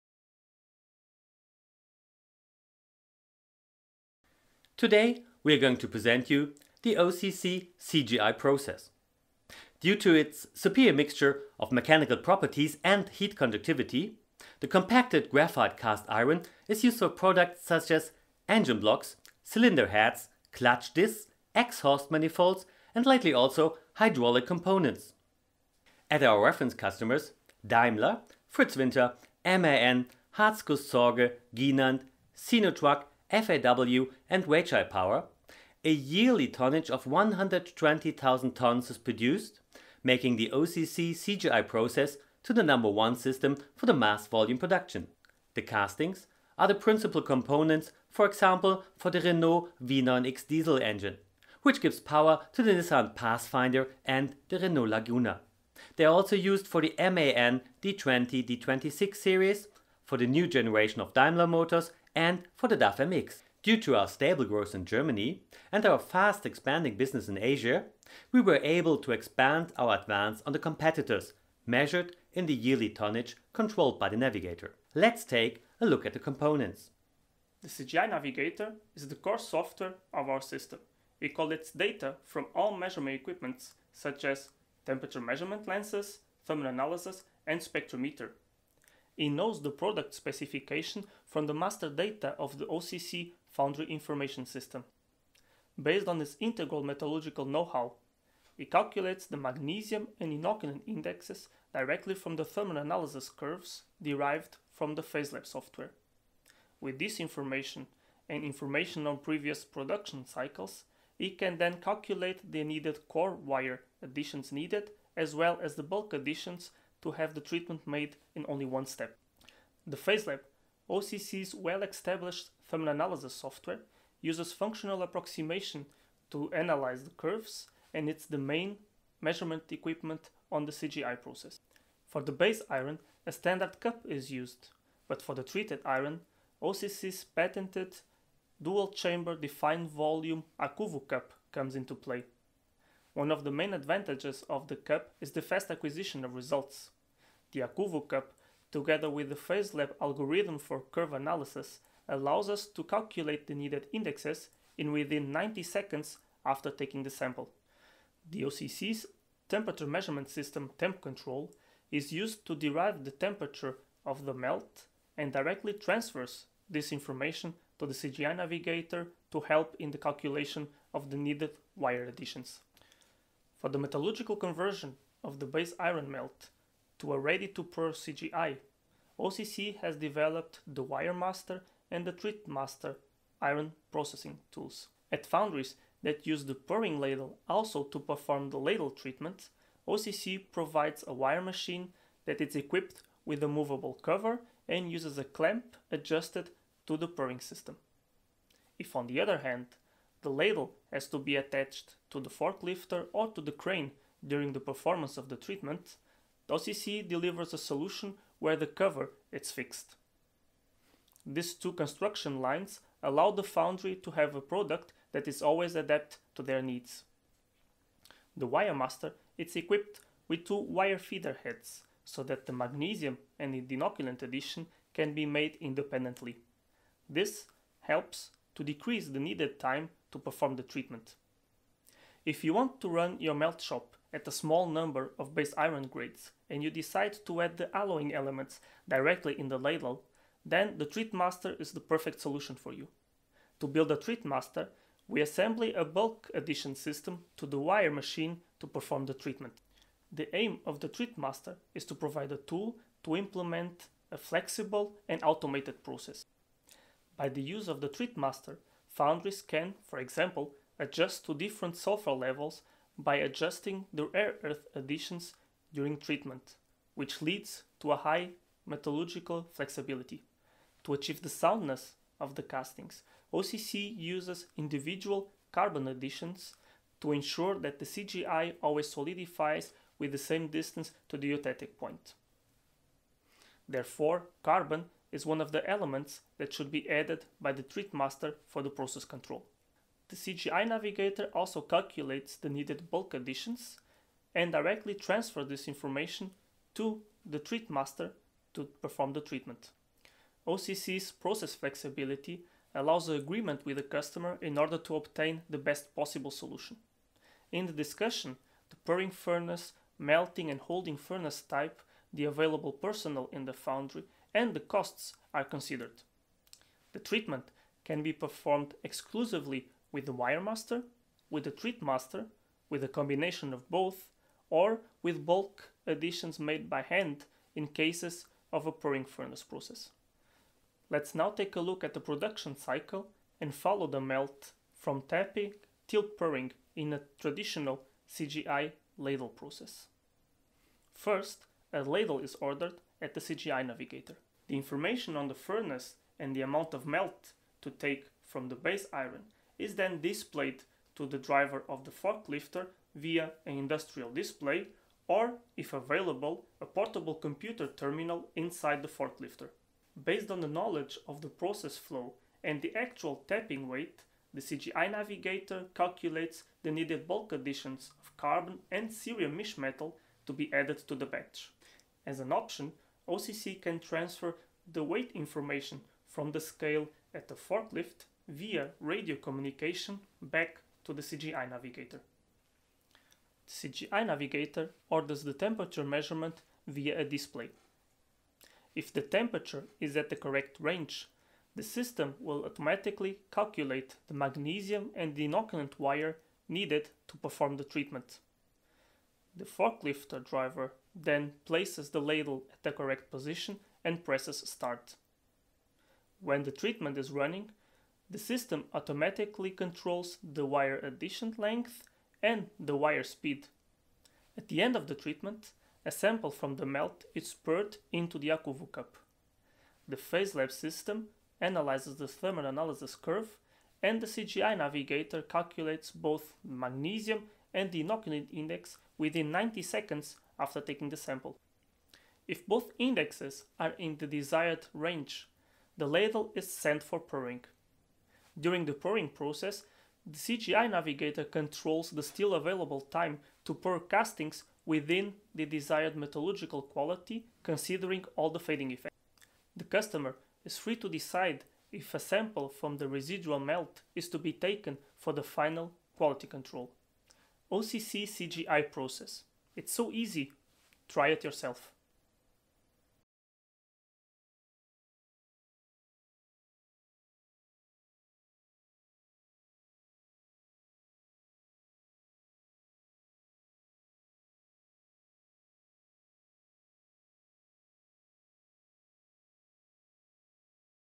The OCC CGI Process
Video presentation of the complete OCC CGI Process.

Including:
- Thermal analysis equipment
- OCC Dual-Chamber-Cup AccuVo®
- Thermal analysis software PhaseLab
- OCC CGI ladle process
- OCC CGI furnace process
- Wire feeder and magnesium treatment system WireMaster
- Dosing system BulkMaster
- Combine system TreatMaster for dosing and magnesium treatment